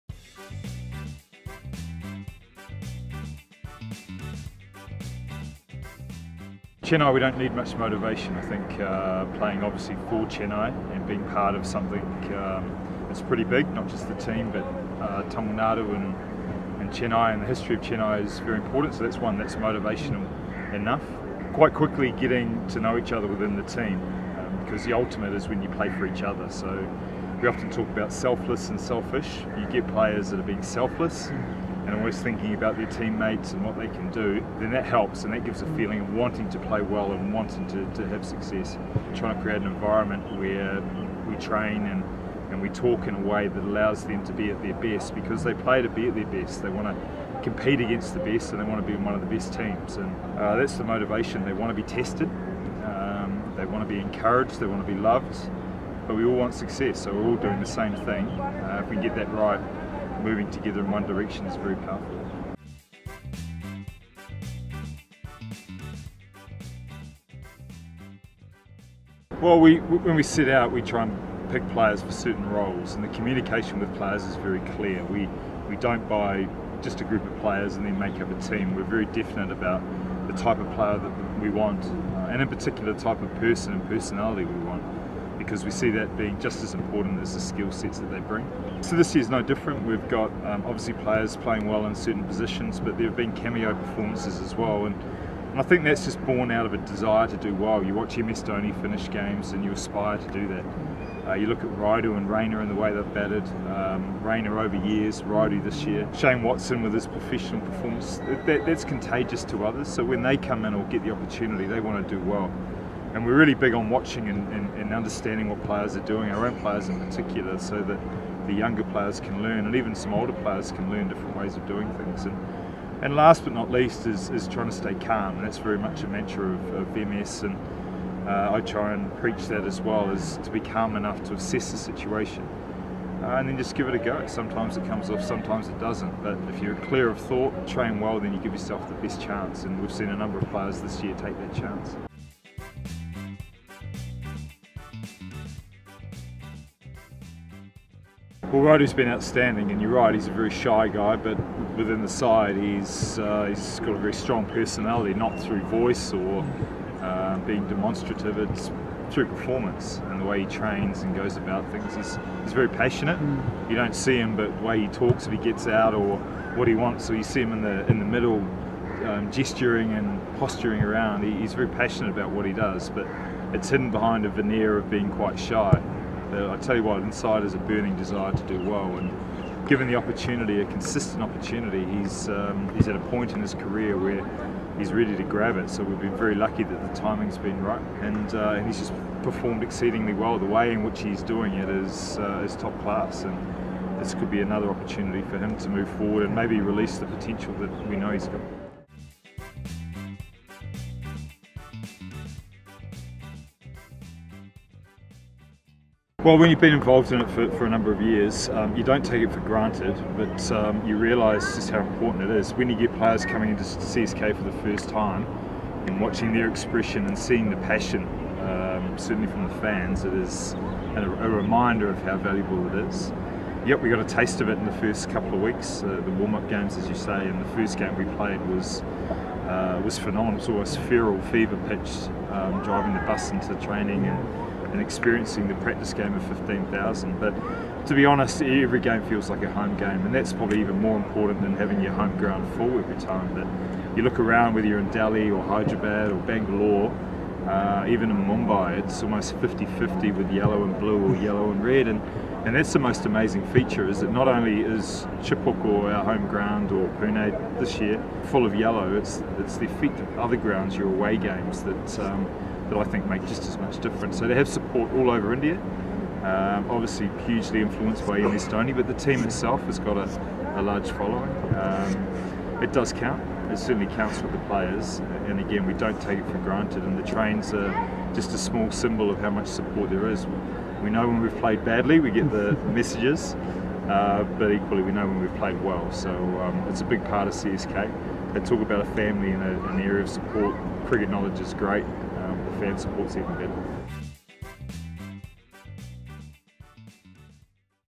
Super Coach Speaks - 2018
As the last league match is about to be staged, Super Coach has a special message to all the fans who have been great support through the season! Don't miss the Rayudu mention too. WhistlePodu Yellove 💛🦁